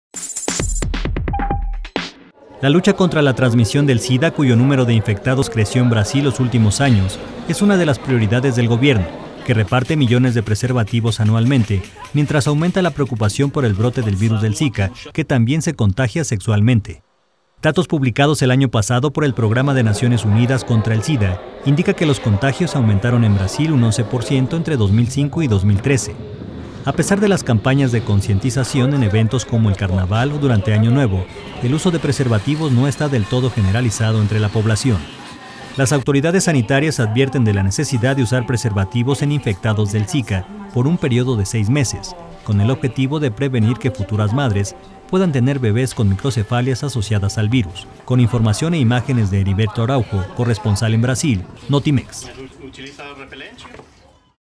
En Brasil buscan crear conciencia sobre uso del condón
La lucha contra la transmisión del Sida y el Zika es una de las prioridades del Gobierno brasileño, que reparte millones de preservativos anualmente